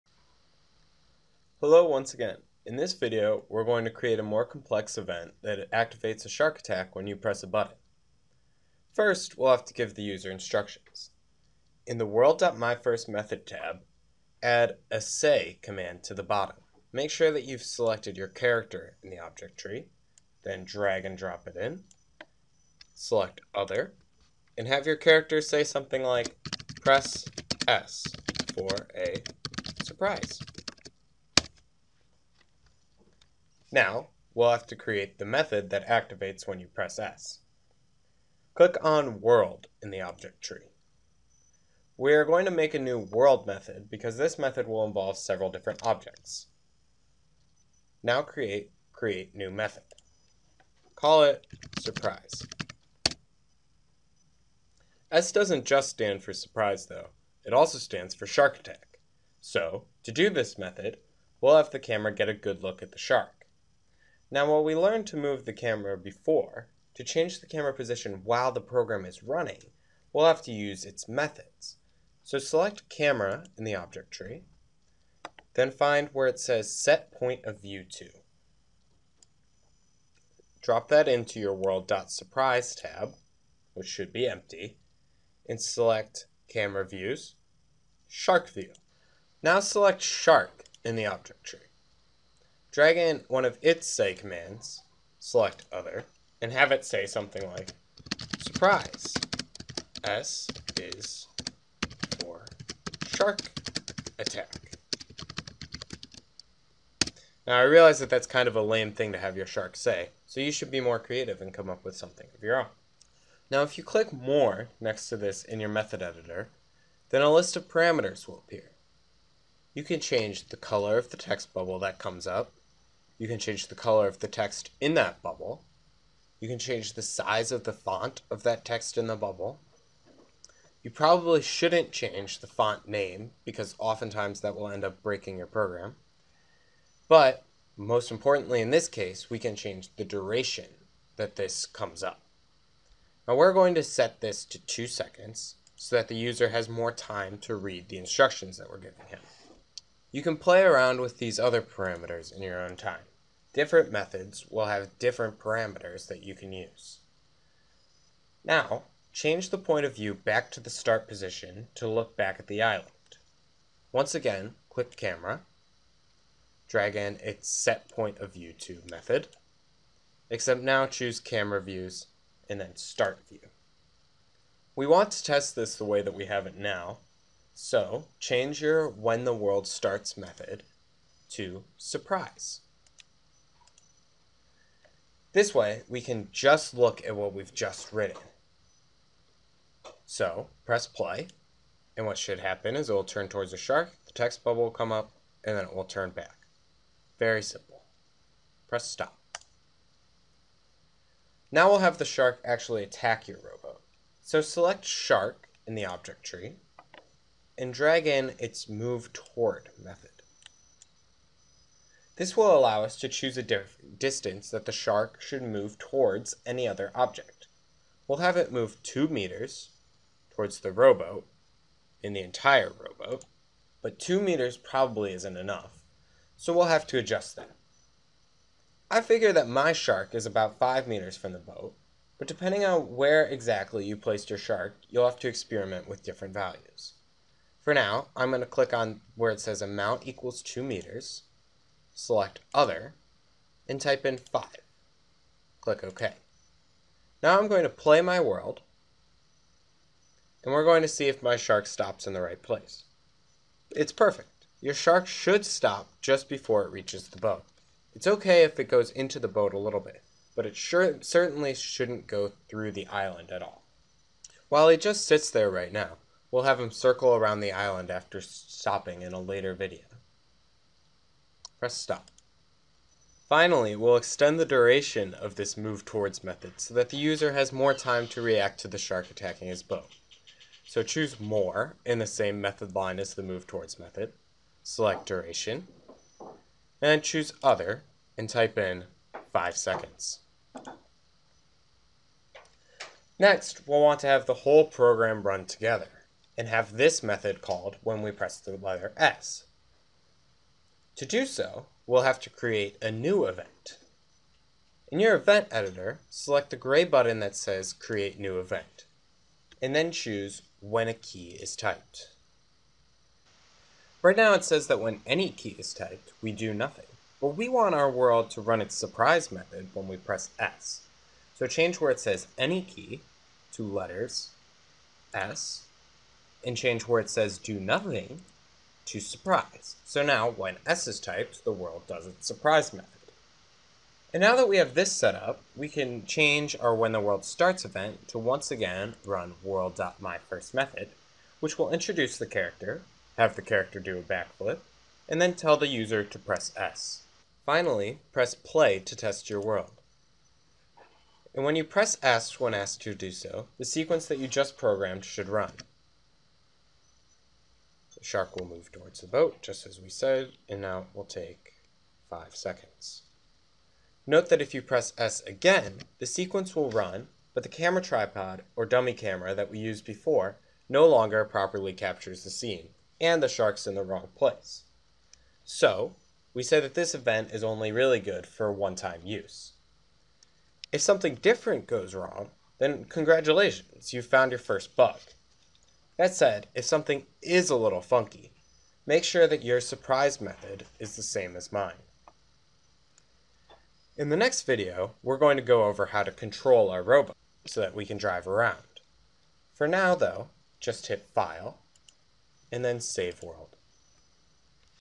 Shark Attack! An Introduction to Programming in Alice (4 of 6)
This video will teach you how to write an "event" to make our shark attack our hero's boat when we press the letter 'S.'
In the full video series, we will teach you how to program a simple world in Alice in which a character does a flip on an island, and then is attacked by (and escapes from!) a shark.

For more 100% free tutorials, including many on more advanced functions in Alice,

Alice is a drag-and-drop object-based programming environment developed by a research team at Carnegie Mellon University.  The skills and thought paradigms you learn here can be extended to studies of more difficult programming languages.

For this video series, you will need to download Alice 2.3

Alice 3.1 is not a simple upgrade to Alice 2.3, and you will not be able to follow this video in using it.  However, we recommend taking a look at Alice 3.1 in your own time, as it has many of its own merits, including better graphics in a more object-oriented approach.